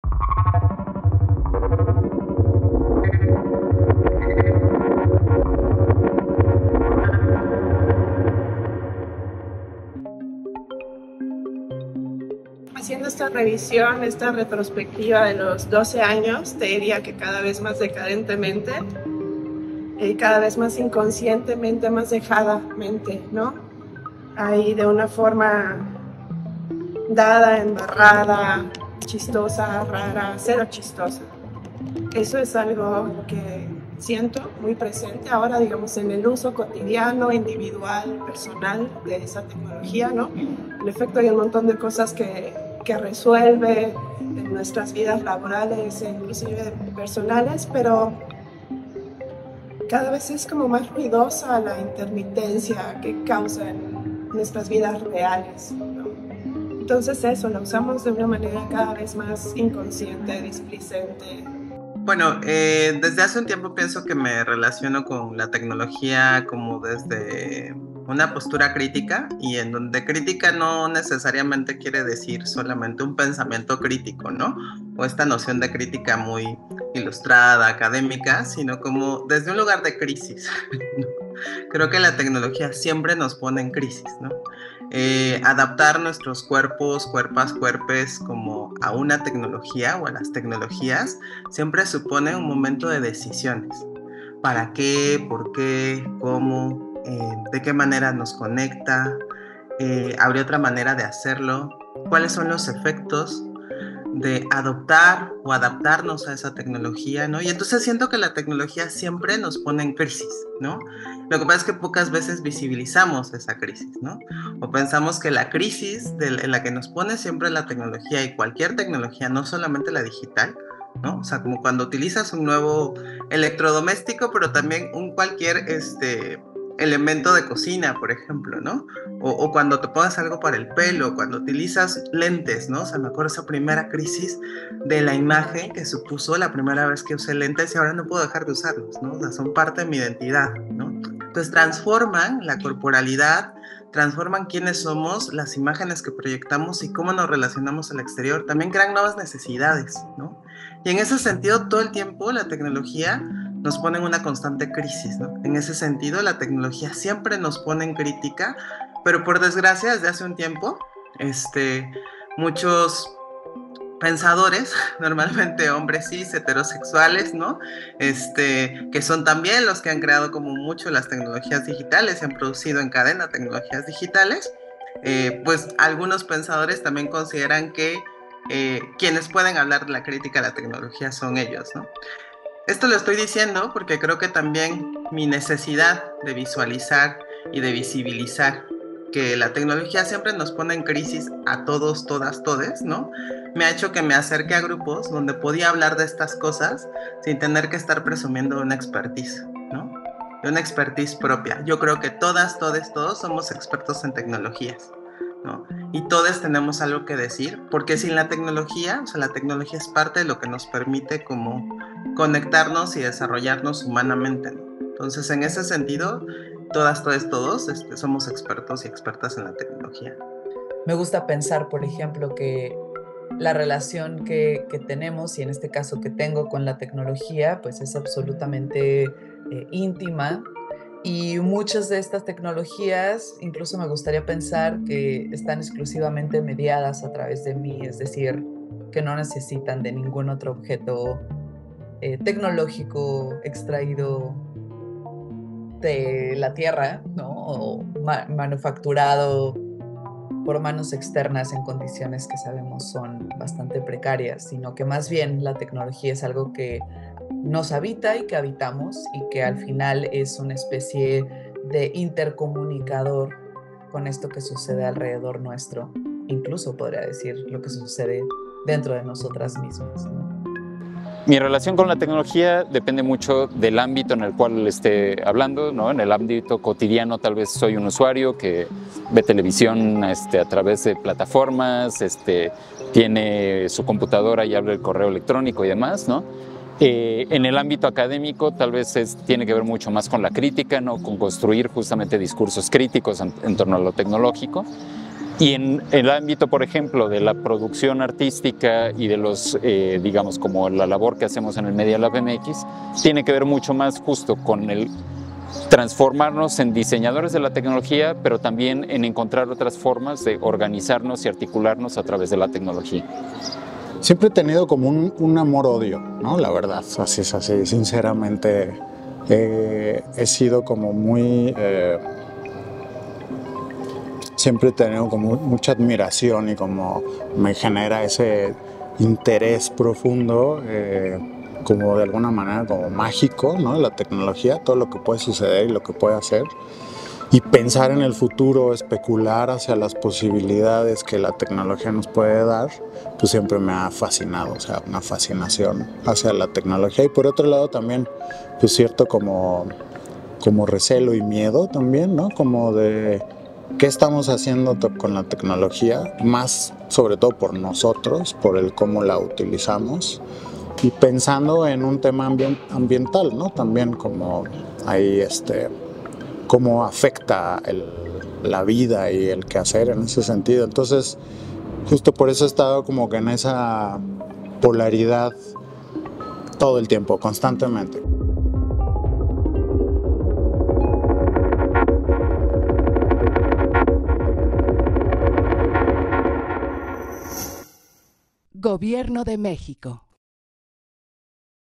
PENSAR JUNTAS #4: ¿Cómo usamos y nos relacionamos con la tecnología?
Trece años después de nuestra inauguración, convocamos a una parte de la comunidad que ha dado vida y forma a este proyecto para reflexionar el peso específico de contar con un Centro de Cultura Digital en la Ciudad de México.

«Pensar juntas» es una conversación polifónica que hace eco de las preguntas que compartimos.

00:00 Mariana Delgado | Gestora
01:02 Nadia Karina Cortés | Investigadora independiente
03:40 Doreen Ríos | Curadora
04:31 Leonardo Aranda | Artista multimedia
05:14 Ary Ehrenberg | Artista multimedia